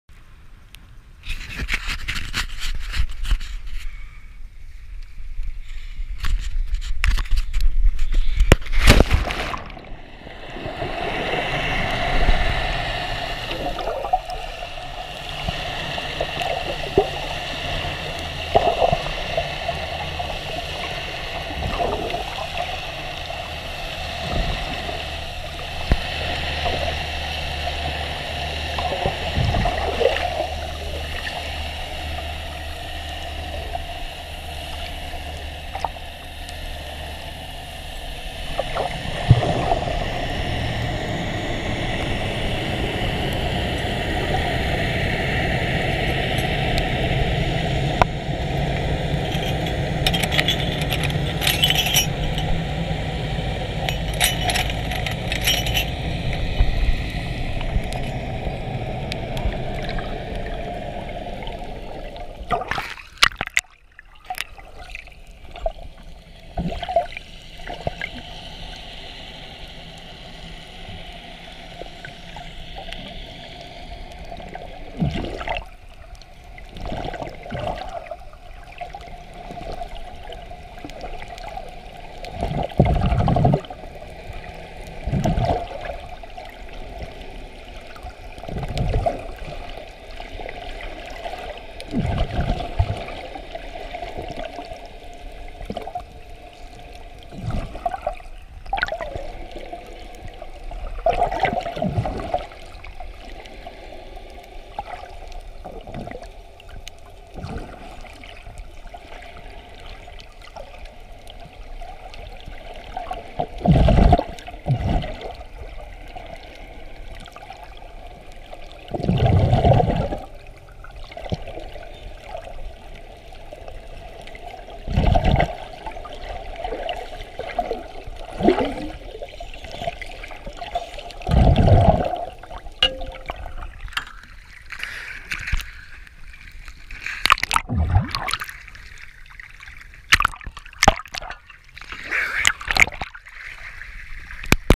Mooring Chain, Vessup Bay, Redhook, St Thomas, US Virgin Islands
My mooring in Vessup Bay, outside Redhook, St Thomas, US Virgin Islands.    I have to maintain the gear.   Three sand screws on the ocean floor, 15 feet deep, with 3 chains that link up to the main riser.    The chains had become corroded and weak, and needed replacing.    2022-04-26